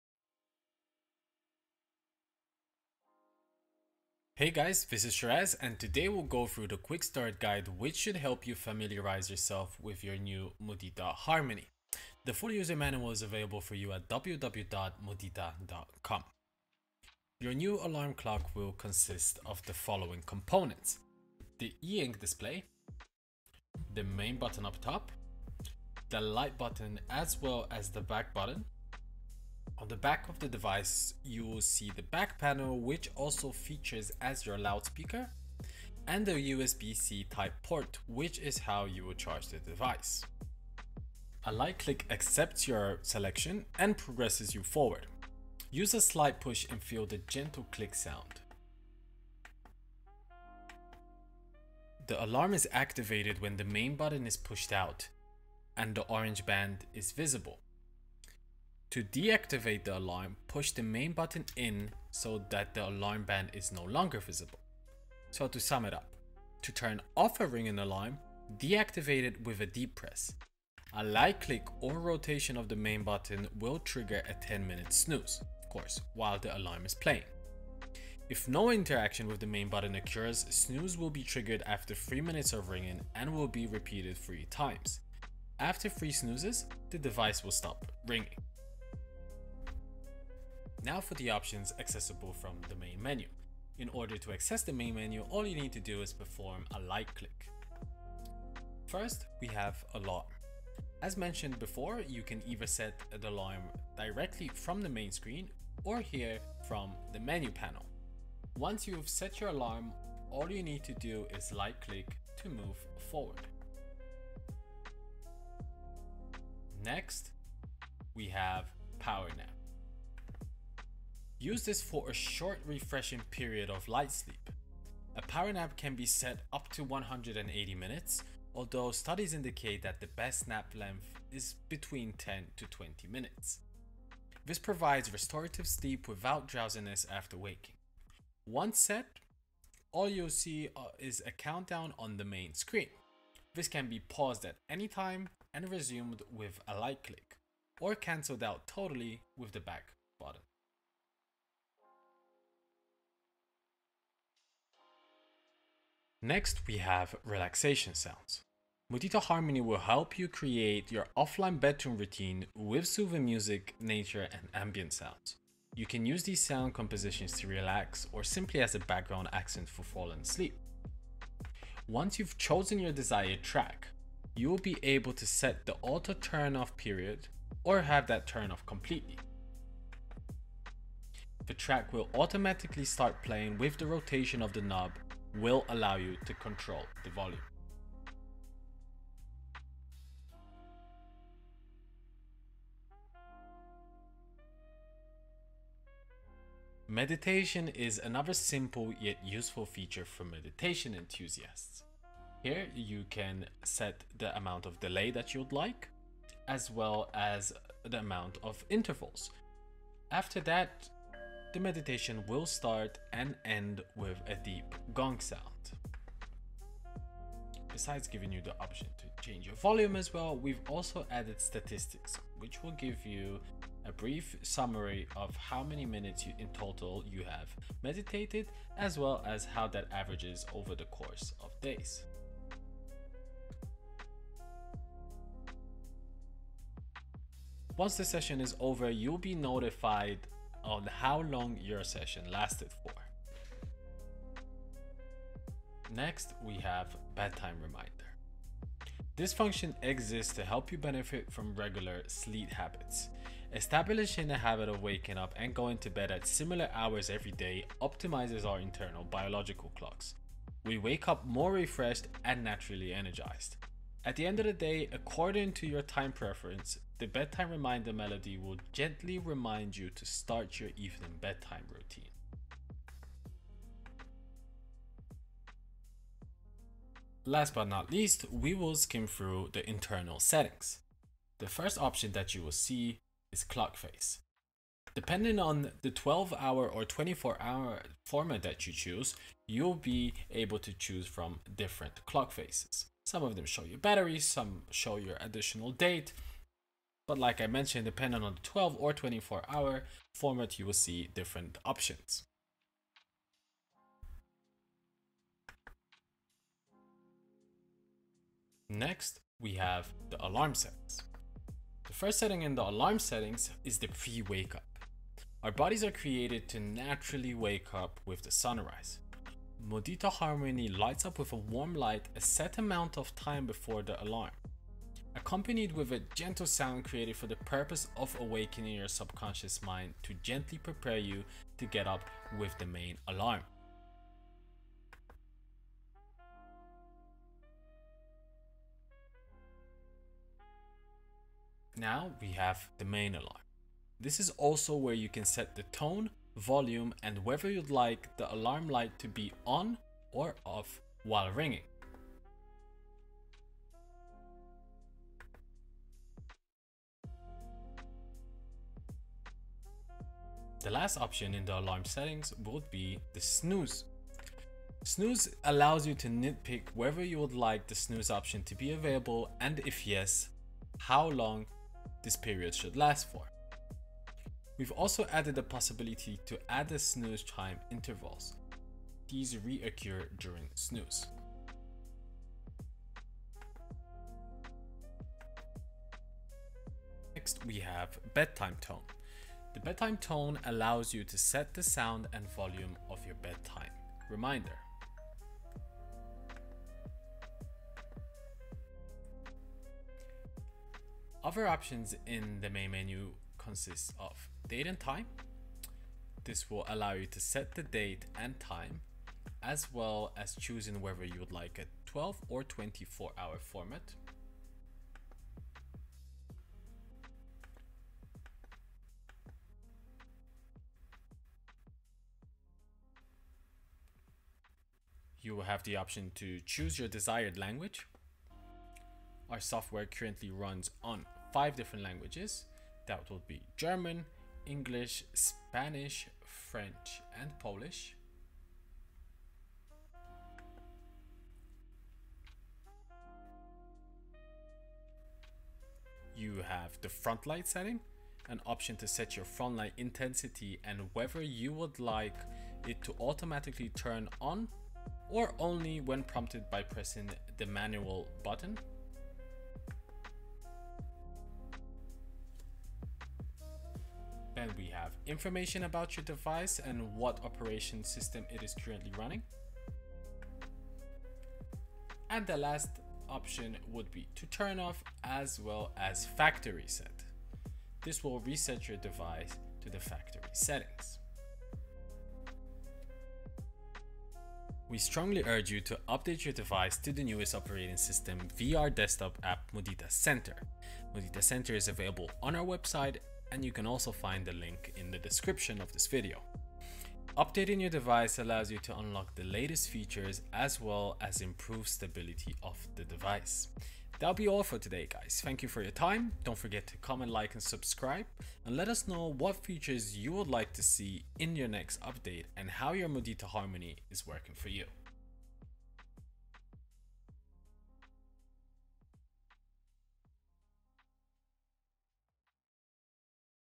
Mudita Harmony - Quickstart Guide
Check out this easy-to-follow tutorial for Mudita Harmony, where you can see how to set up your device & use the features of this minimalist alarm clock. It's super easy to follow and a perfect way to learn how the device works.

Mudita Harmony is your sleep companion which helps you establish healthy bedtime habits, improve the quality of your sleep, as well as bring you a sense of calm. Remove your phone from the bedroom and wake up to carefully crafted sounds without all the unnecessary distractions and information overload.

We're big fans of E Ink displays, as they don’t cause eye strain or fatigue and have a natural, paper-like, and analog feel. Our modern, 2.84” E Ink display, with a resolution of 480x600, a PPI of 270, and support of 16-bit grayscale, makes the clock screen easy to read in normal light. Relaxation is the most underrated part of health and well-being.

As the world becomes more and more digital, there's a growing need for us to slow down and take time out of our day to simply relax.

Our mission was to use the most responsible options available. Our every choice reflects the pursuit to provide more fair trade and ethical devices.

00:00 Intro
00:18 What's what
00:42 Navigation
01:41 Setting an alarm
01:57 Power nap
02:34 Relaxation sounds
03:16 Meditation Timer
04:05 Bedtime reminder
04:36 Internal settings
05:12 Pre-wake up
05:41 Main alarm + snooze
06:25 Additional settings
08:04 Update your device!
08:24 Outro